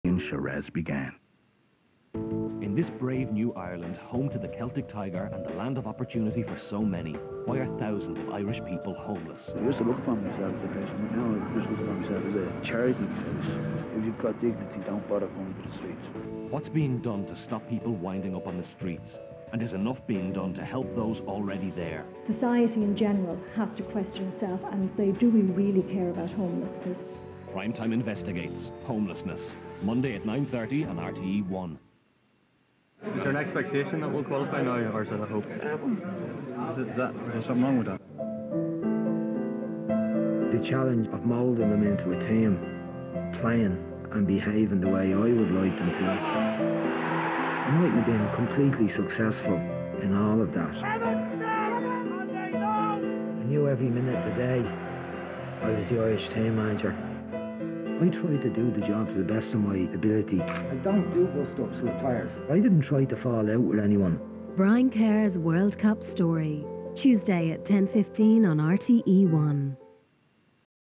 2006 - RTÉ Homelessness Special & Irish Soccer Documentary Adverts
2006. Homeless crisis in Ireland. National side failing to qualify.

Some things never change, eh?